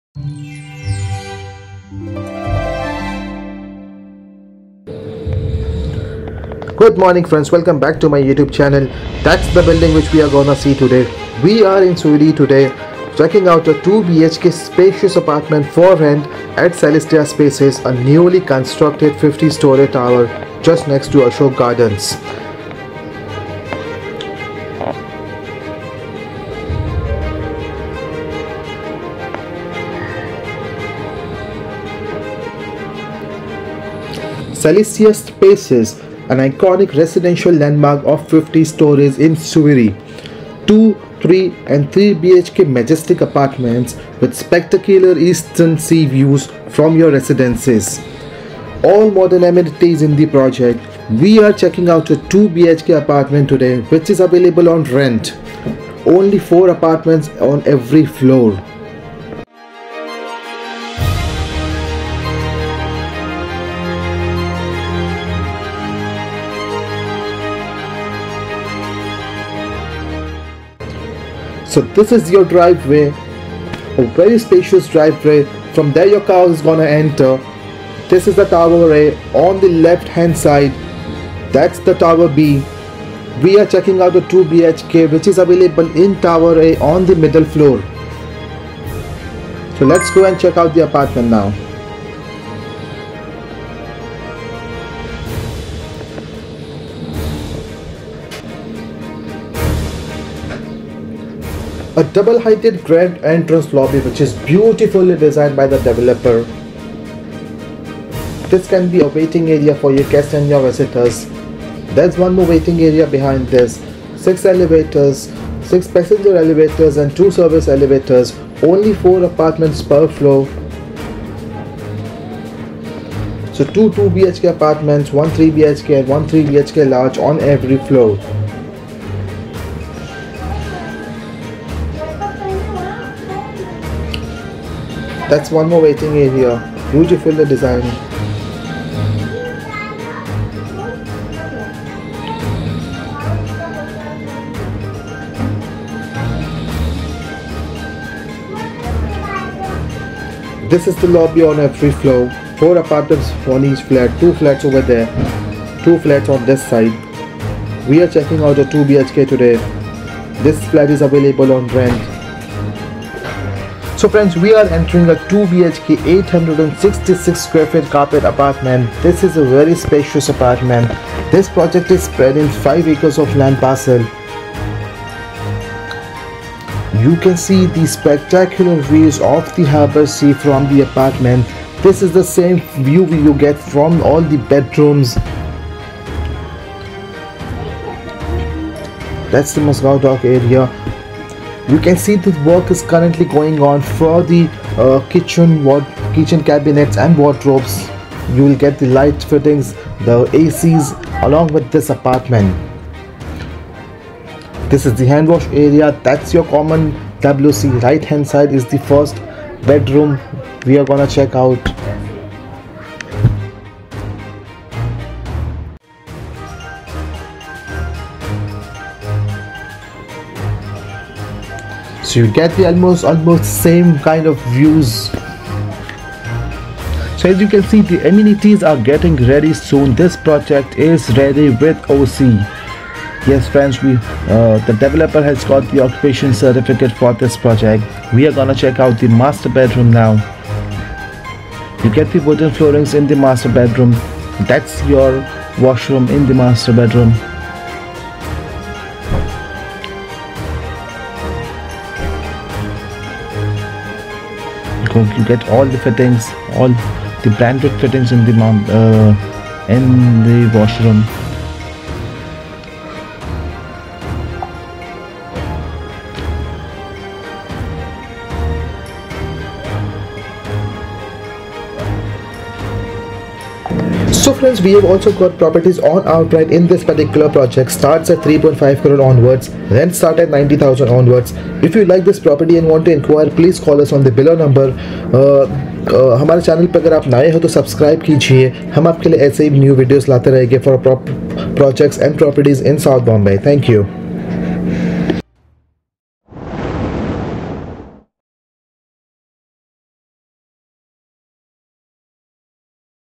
90k Rent with Sea Views☺️ 2 BHK in Celestia Spaces, Sewri. Sea Views & 90k Rent Only😱
Hello Friends,

We are marketing a 2 Bhk flat on Rent in Celestia Spaces Sewri, This is a ready to Move in Project with all modern Ameneties located in the heart of South Mumbai.

We also have flats available on outright in this project below are the configs:

2 Bhk - 866 Carpet
3 Bhk - 12333 Carpet
3 Bhk  Majestic - 1468 Carpet.